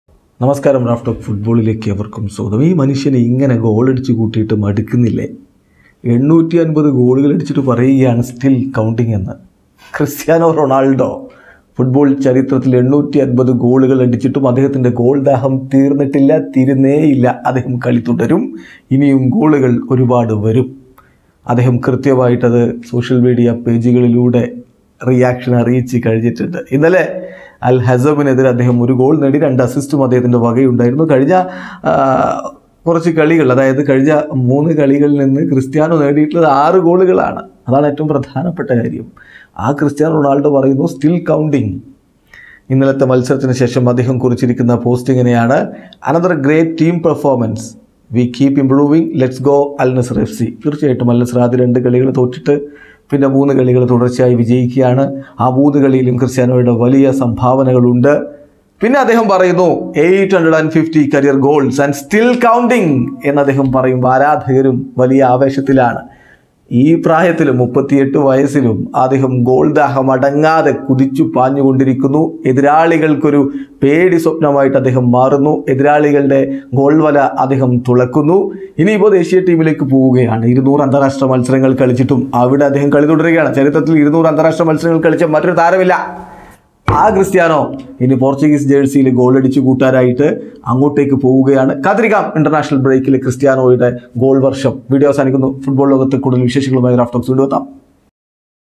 Still counting!😉💪🏼: 850 ഗോളുകൾ നേടിയ ശേഷം CR7ന്റെ പ്രതികരണം | Cristiano Ronaldo
Al-Hazm vs Al-Nassr
Cristiano Ronaldo